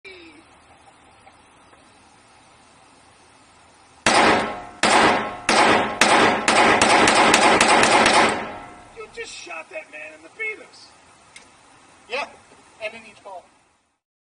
Me and Lou, Fox Valley Shooting Range, Rock Island M1911 XT22M, .22 Magnum
Shooting at a moving target from 27 to 10 feet,